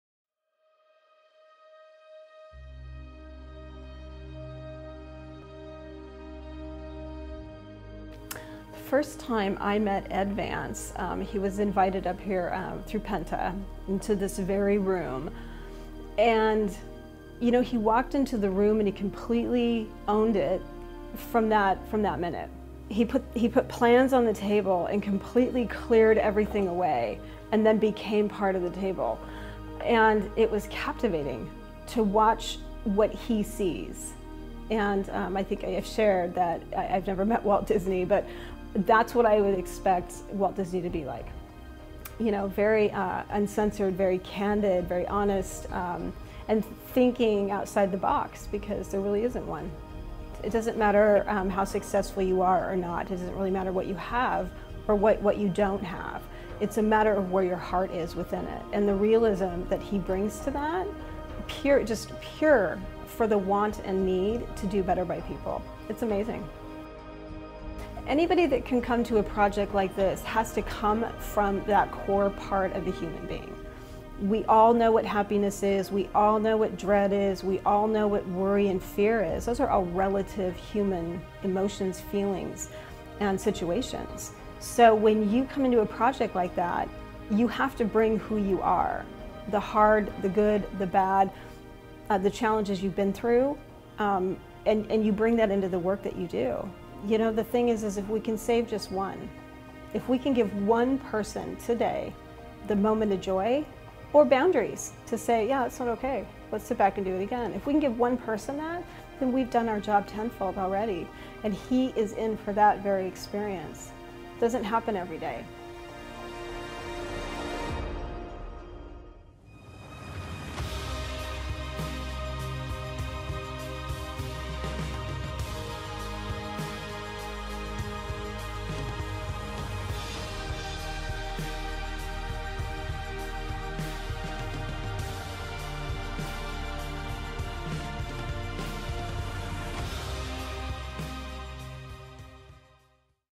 EV&A | Heather Engle Testimonial
Iconic Las Vegas architecture firm, Ed Vance and Associates, receives this glowing testimonial. Heather Engle is the CEO of the amazing Las Vegas Rescue Mission. Heather and her company have helped 10's of thousands of people and served 100's of thousands of meals. This testimonial means the World to Ed and his team.